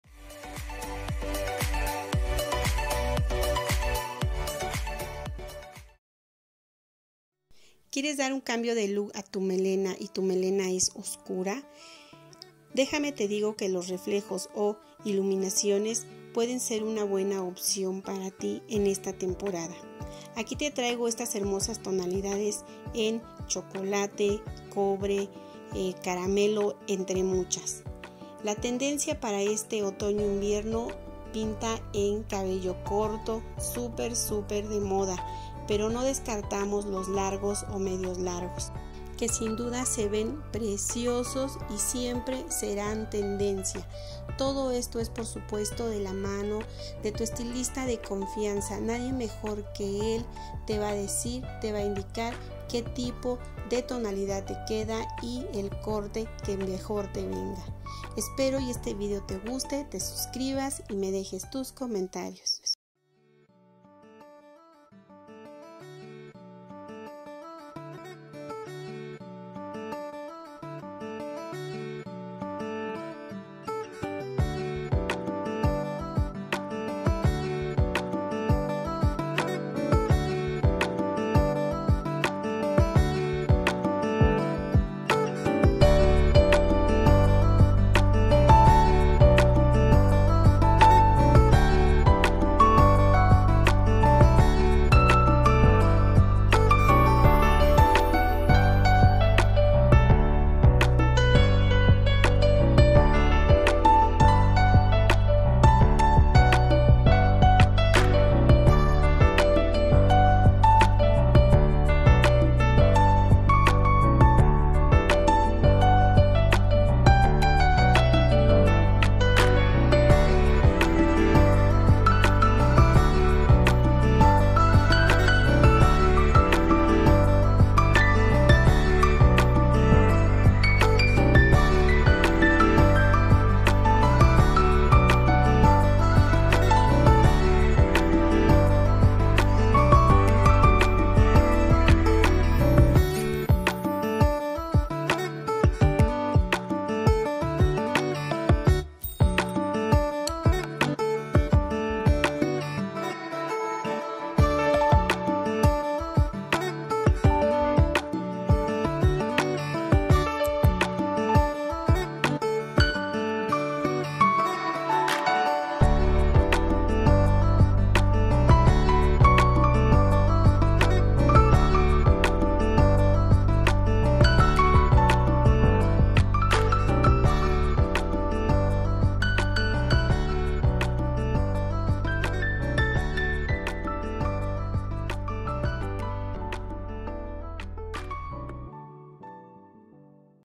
IDEAS CAMBIO DE LOOK/REFLEJOS & ILUMINACIONES PARÁ CABELLO NEGRO 🖤 @todoparamujerbellezamas3163 🌷
Bienvenid@s 🌷

Bellas ideas de un cambio de look a tu cabello. Reflejos e iluminaciones que te harán lucir más bella dando un cambio radical y con estilo.